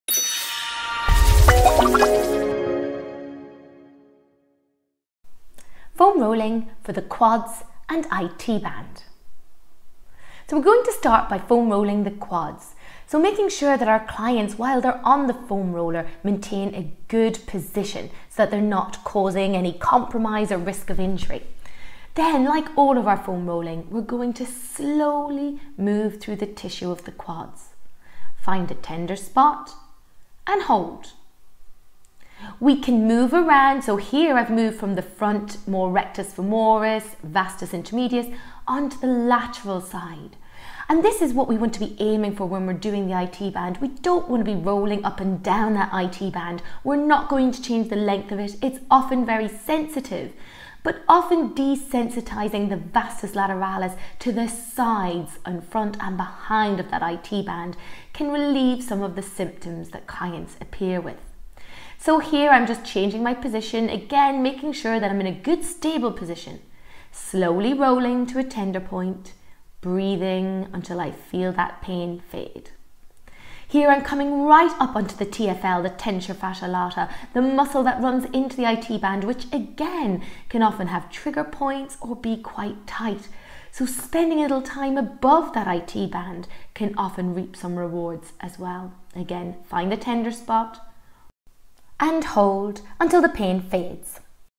Sports Massage Homecare: Foam Roll Quads & IT Band
Sports massage gives relief of pain and reduces tone in the muscles but getting them stronger has longer term results.  Some of our massage techniques can be self administered by the client e.g. soft tissue release, massage ball work & foam rolling.  Here we target the quadriceps and IT band area.